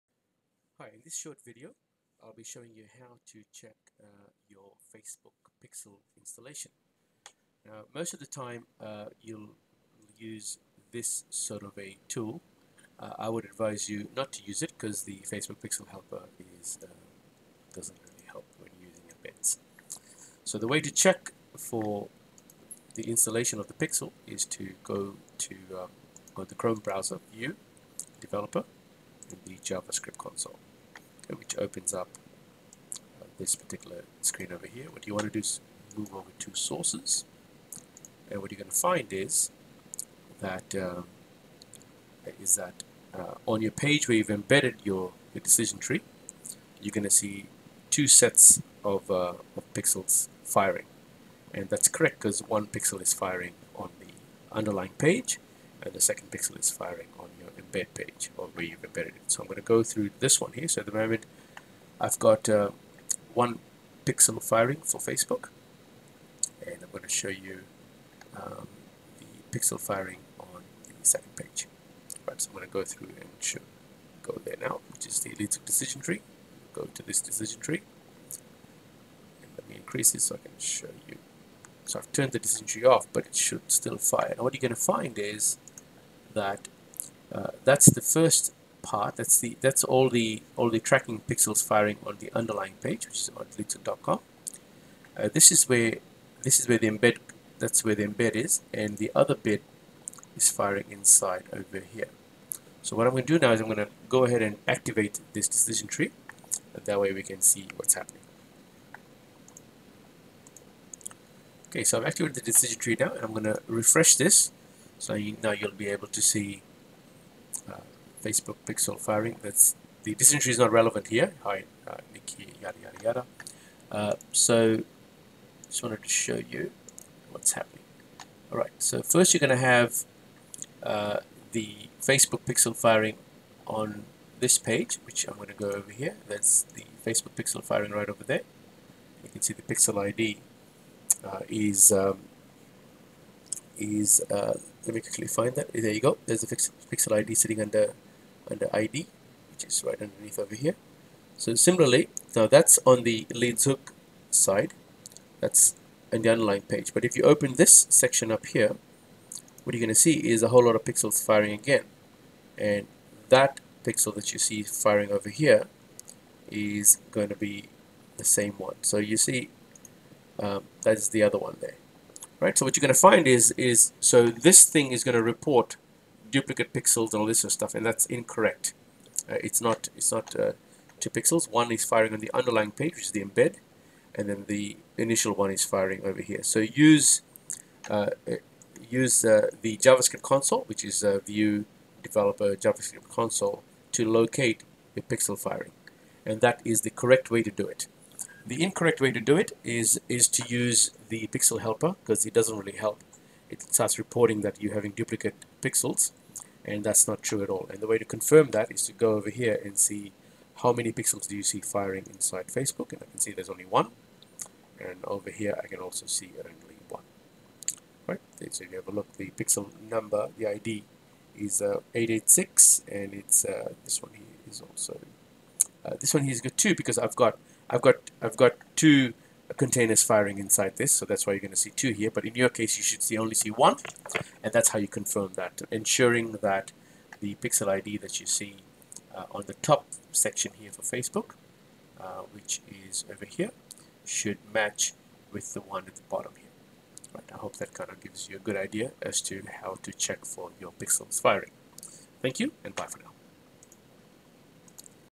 How to Properly Check If Your Facebook Pixel Is Firing
In this short video you'll learn...

* How to Check Your FaceBook Pixel installation when tracking embedded content.

* When the FaceBook Pixel Helper Extension is not very helpful.